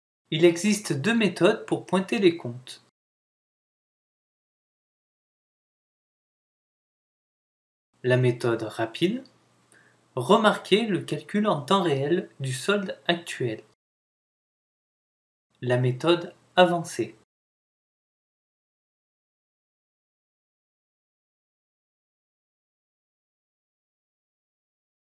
Pointer les comptes bancaires - Logiciel Comptabilité Personnelle
Cette vidéo montre comment pointer les comptes bancaires.